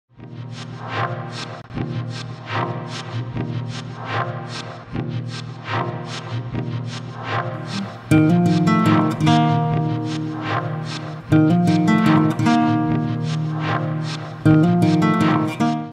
Kinect+processing and cinema 4D music animation
A video with kinect, processing and cinema4D for university.
by Luis Almeida and Daniel Cardoso